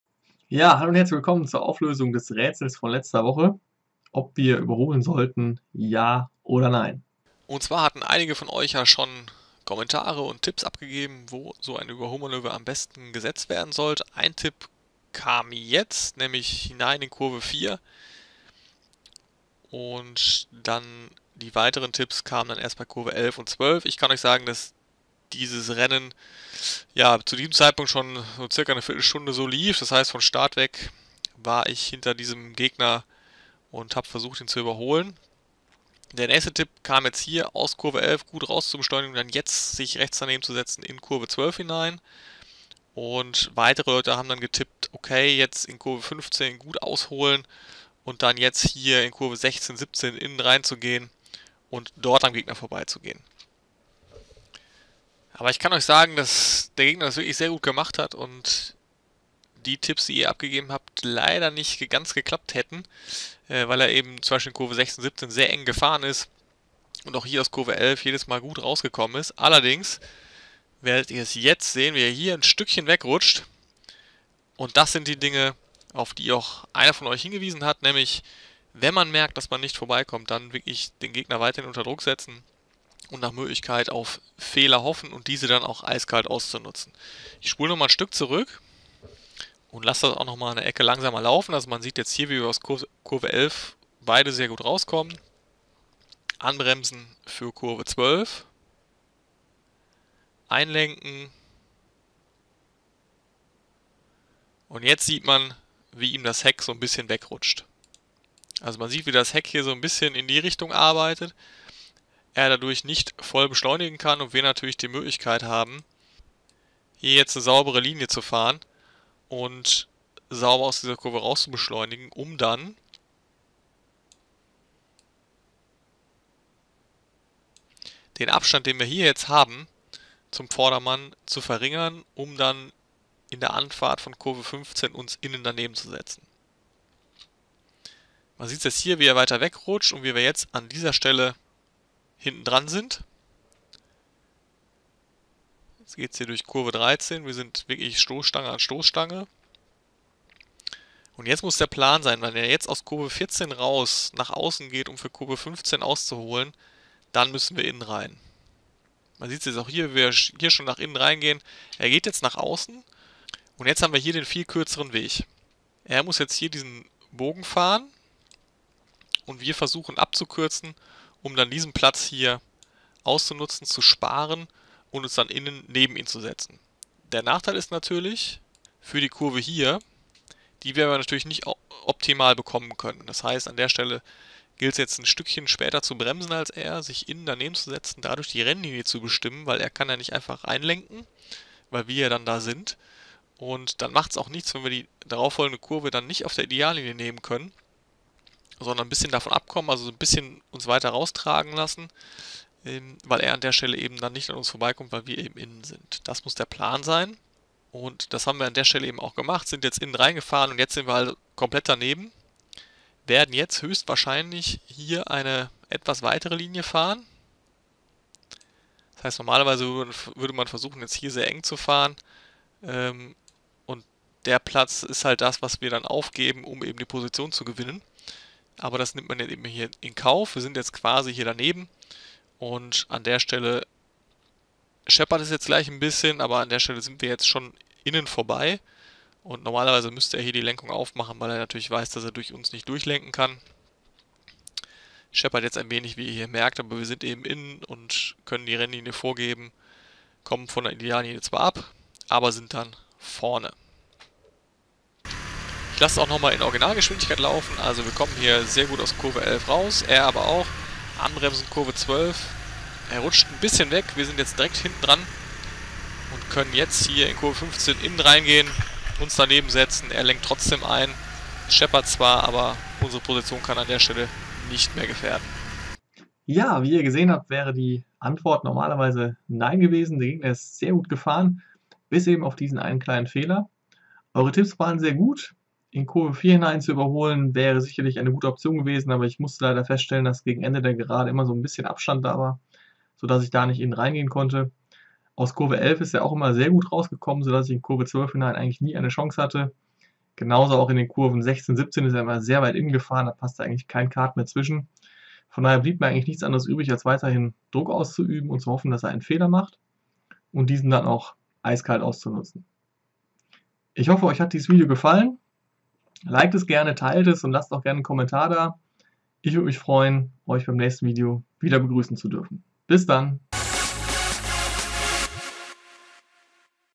Auflösung des Rätsels: Überholen? Ja oder nein?
In diesem Video hatten wir gefragt, ob ihr den Gegner überholen würdet und wenn ja, wo
Hier kommt nun die Auflösung.